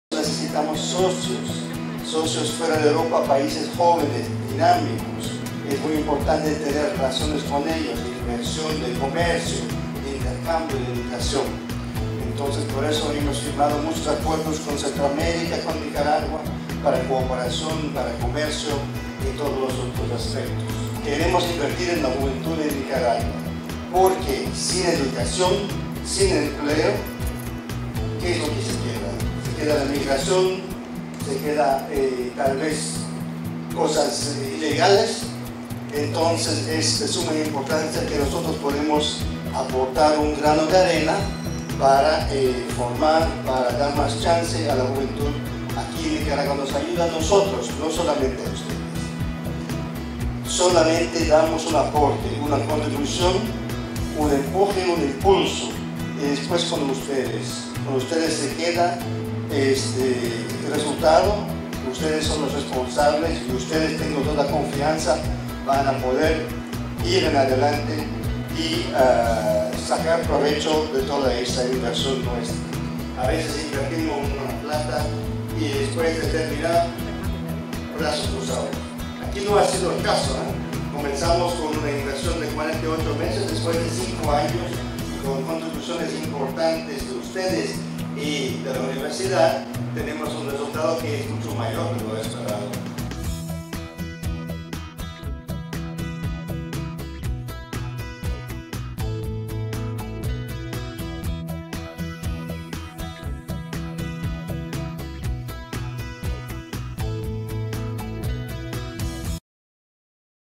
Embajador de Unión Europea comenta sobre Nicaragua
El señor Kenny Bell estuvo presente en la inauguración de nuestro proyecto CEDEAGRO y comentó la importancia de Nicaragua para la Unión Europea